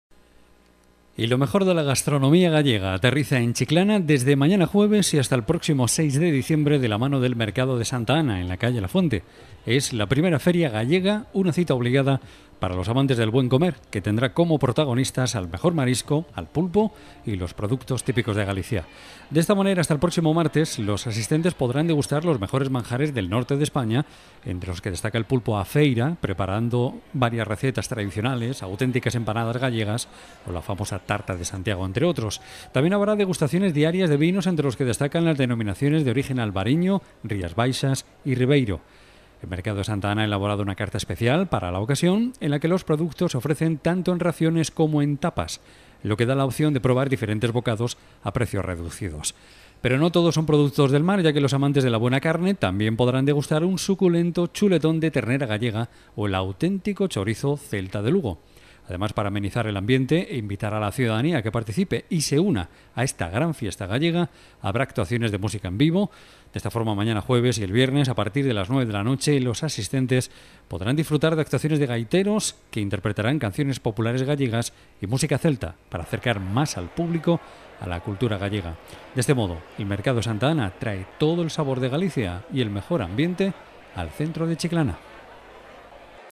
FERIA GALLEGA
• Lo mejor de la gastronomía gallega aterriza en Chiclana, desde mañana jueves y hasta el próximo 6 de diciembre, de la mano del Mercado Santa Ana, en la calle La Fuente. La I Feria Gallega tendrá como protagonistas al mejor marisco, al pulpo y los productos típicos de Galicia.